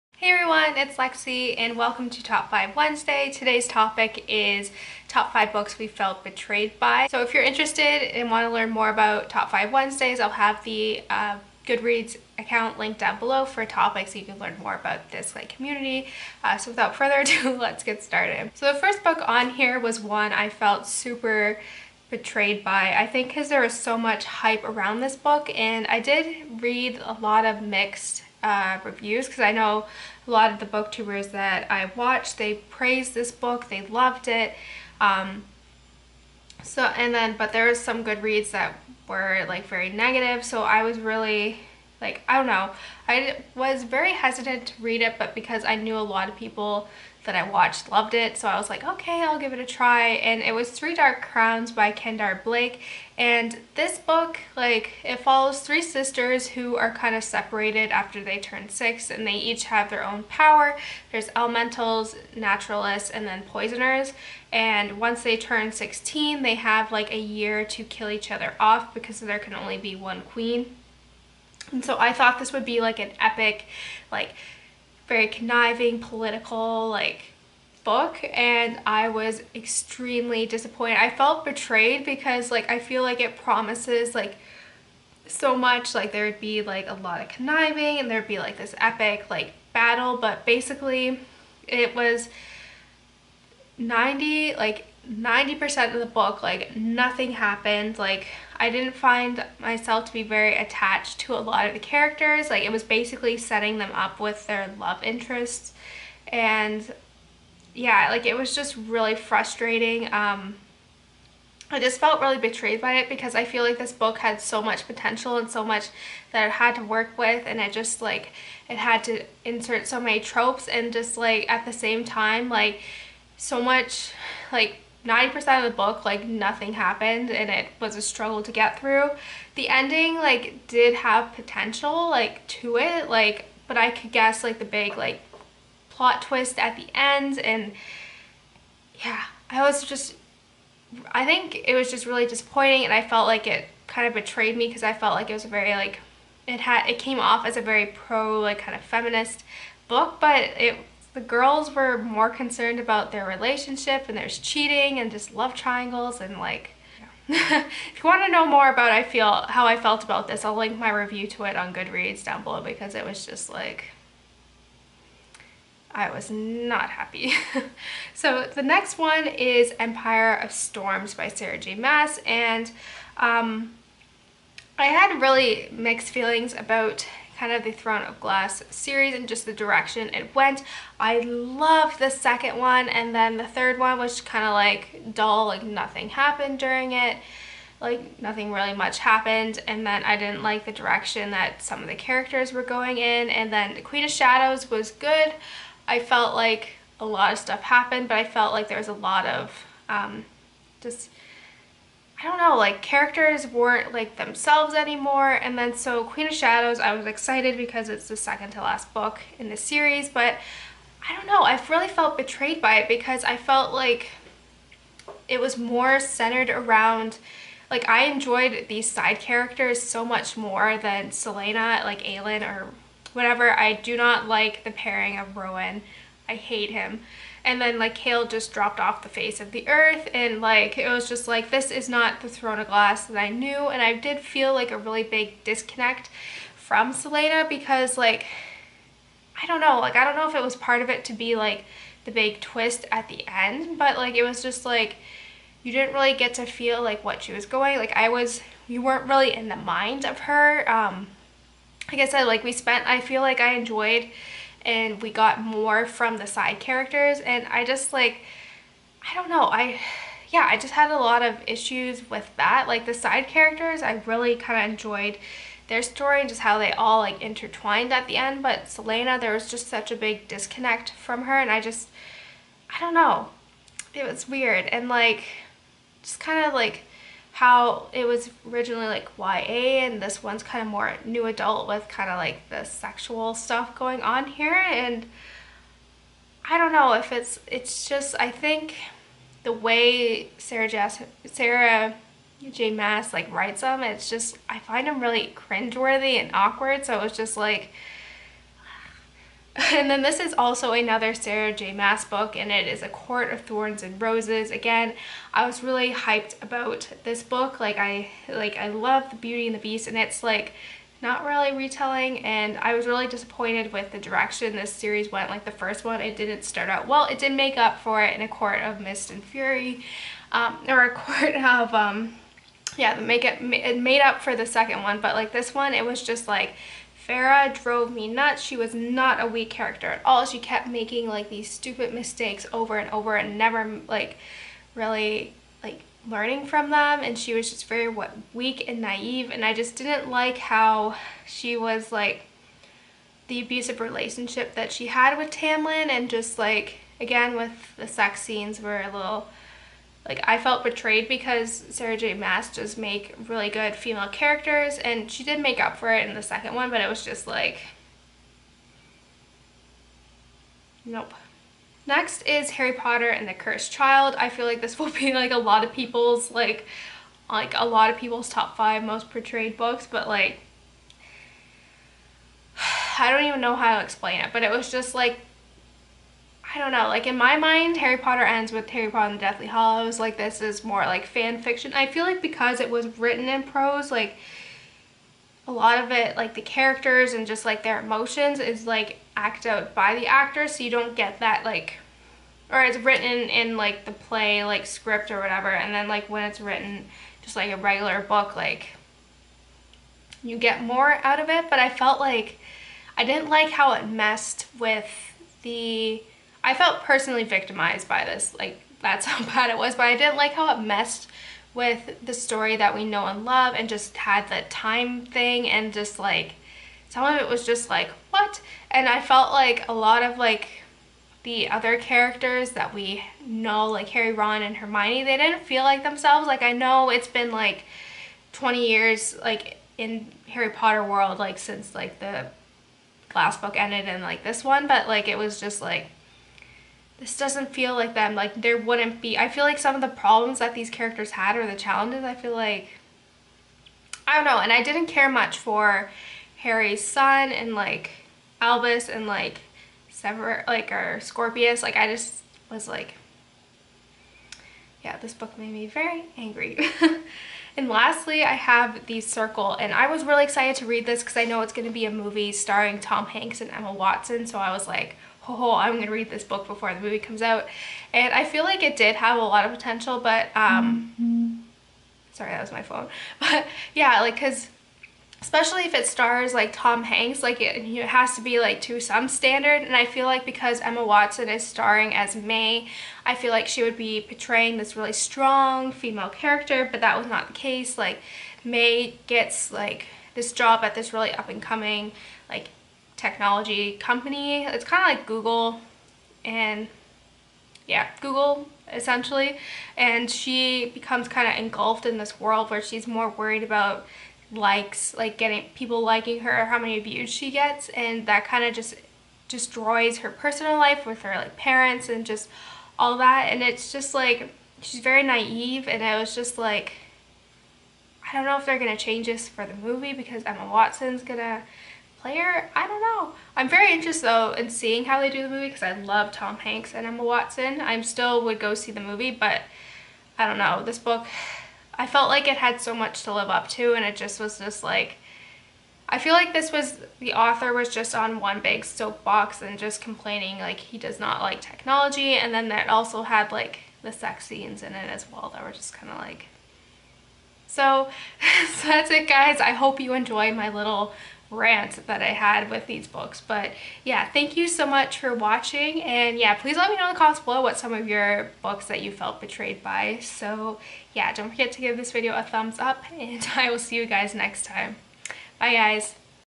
Books I Felt Betrayed By | T5W
---Books Mentioned
- Three Dark Crowns by Kendare Blake
- Empire of Storms by Sarah J. Maas
- A Court of Thorns and Roses by Sarah J. Maas
- Harry Potter and the Cursed Child
- The Circle by Dave Eggers